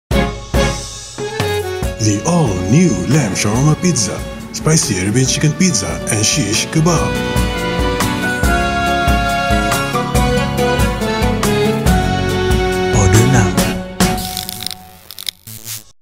🌙 Savor Ramadan Delights: Spicy Arabian Chicken Pizza, Lamb Shawarma Pizza and Shish Kebabs!🍕
Celebrate Ramadan with our special dishes! Enjoy fiery Spicy Arabian Chicken Pizza, flavorful Lamb Shawarma Pizza and juicy Lamb Shish Kebabs. 🌙🍴